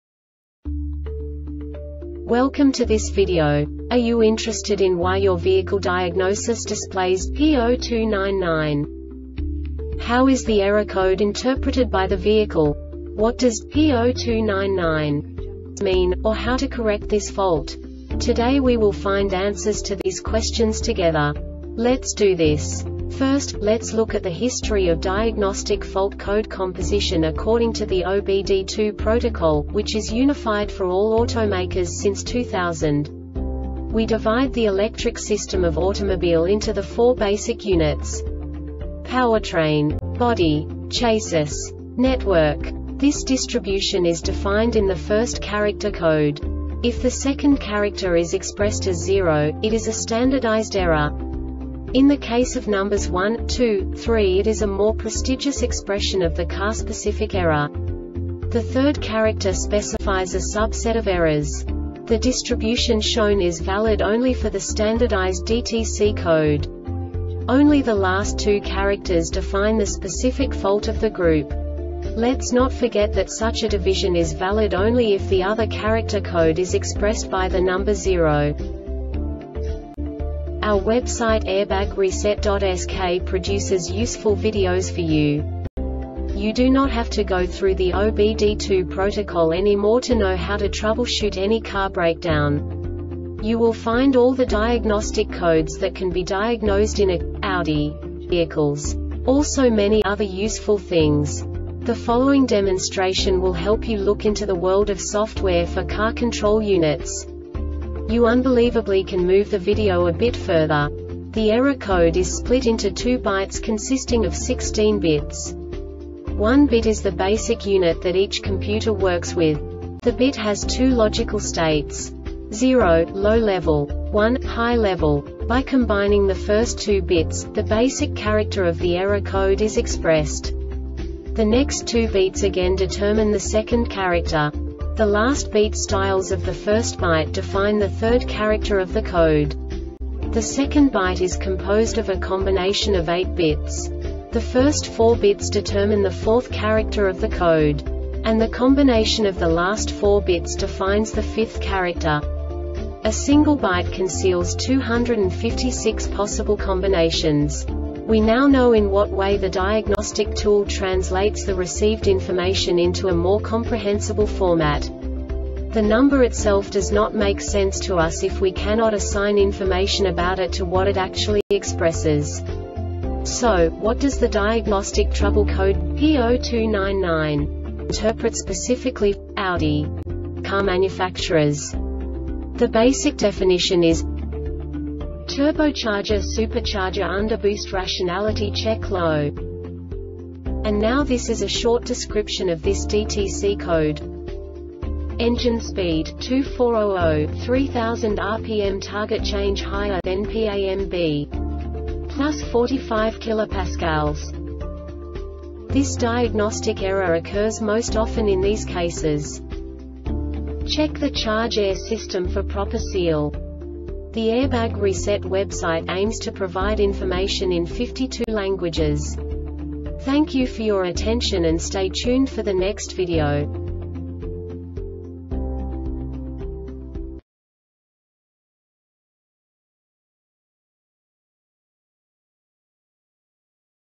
DTC Audi P0299 Short Explanation
Contents:
0:21 Basic DTC analysis according to OBD2 protocol standard.
1:48 Insight into programming
2:58 A diagnostically understandable description of the Diagnostic Trouble Code
3:05 Basic error definition

Make:
Audi
Code:
P0299
Definition:
Turbocharger/Supercharger Underboost Rationality check low
Description:
Engine speed, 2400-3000 RPM target change  higher then P amb. + 45 kPa
Cause:
Check the charge air system for proper seal.